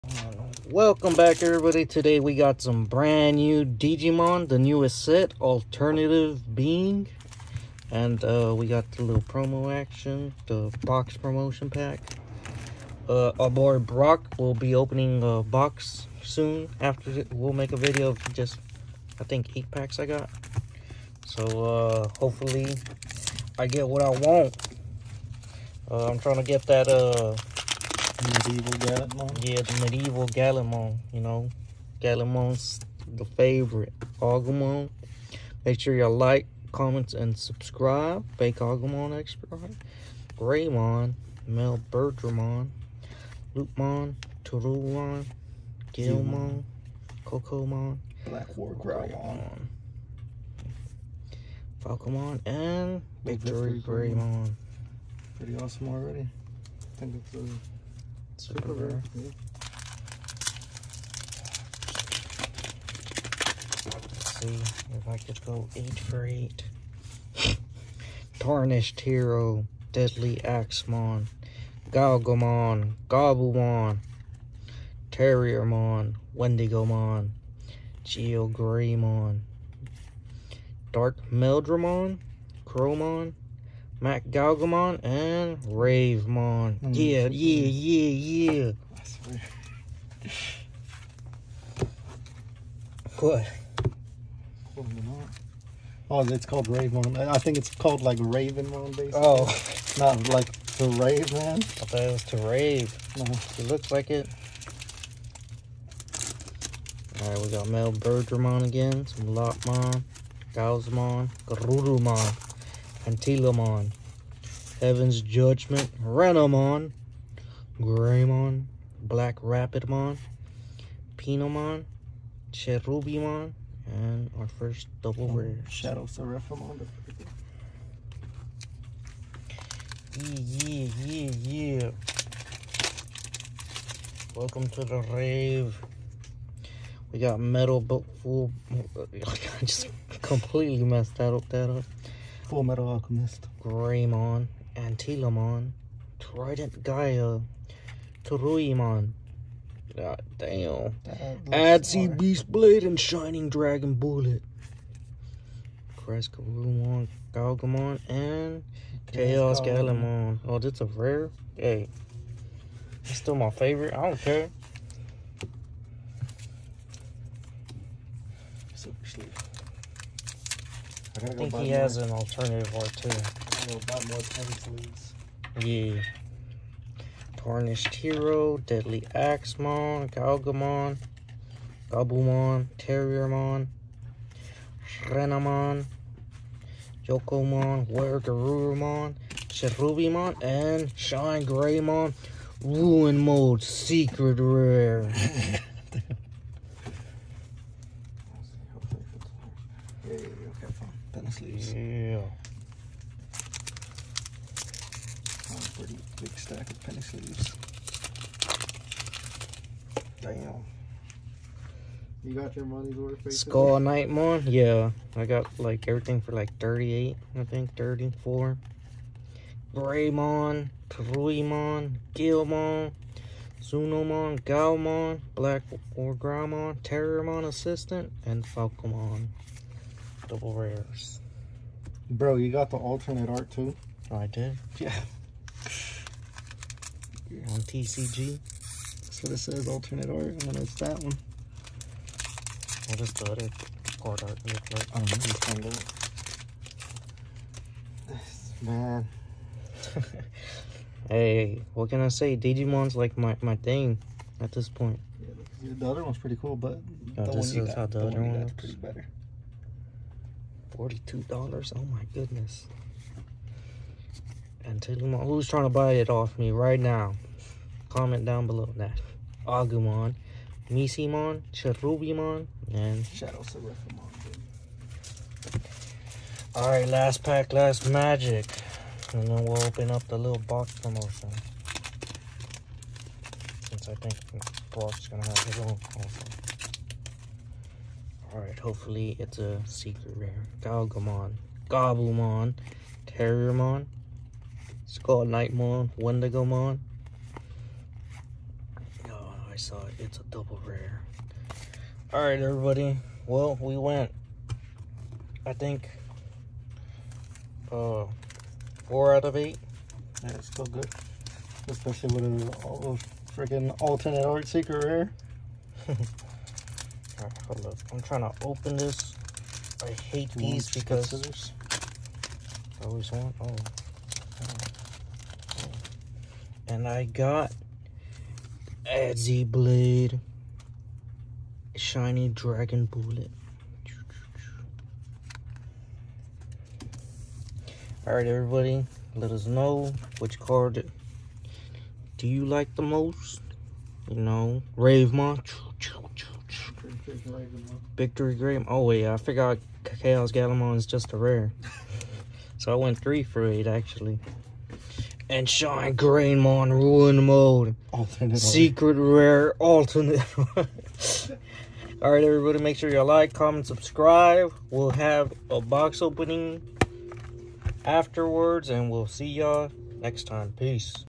NEW Digimon Alternative Being pack openings!! ALT CARD PULLED⭐️🔥
Today Keke opens his booster packs of Digimon’s newest set Alternative Being! He also pulls a alt card! Let’s see what he pulls!